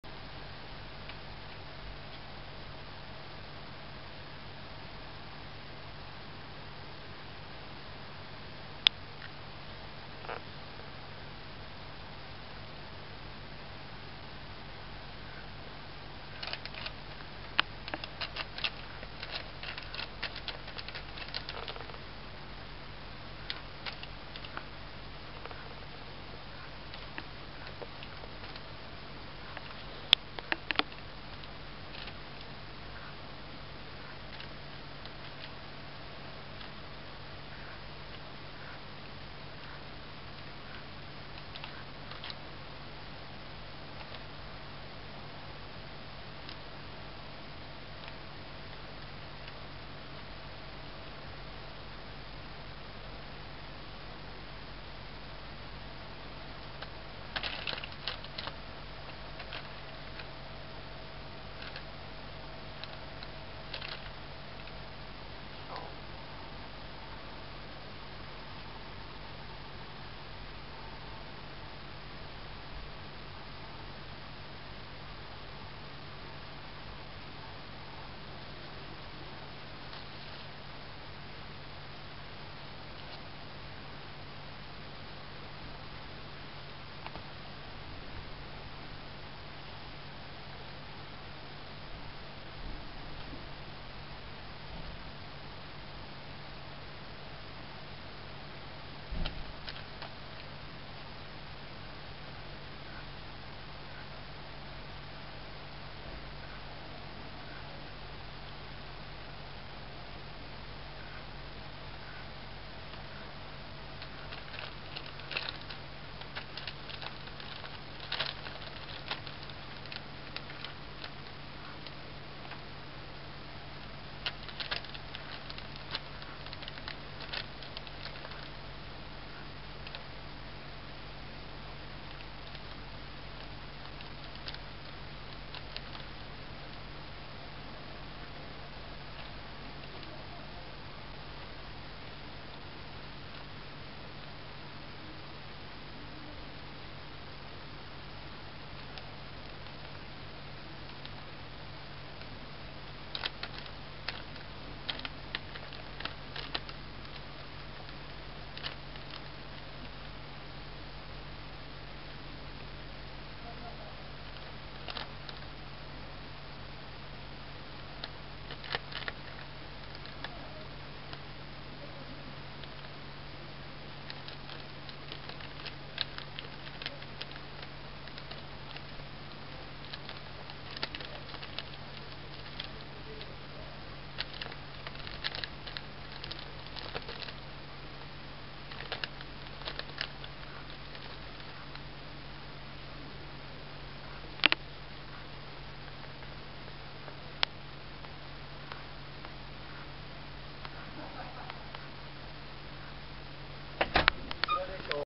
捨猫、ネコパンチを練習する子猫の本能？
このニャンコは、生まれた直後なのか目も開かない状態で路上を彷徨っていたところを保護したニャンコです。猫インフルエンザと診断され一時期はどうなるかと思いましたが、今は元気になり「猫パンチ」から着実にニャンコライフを歩んでおります。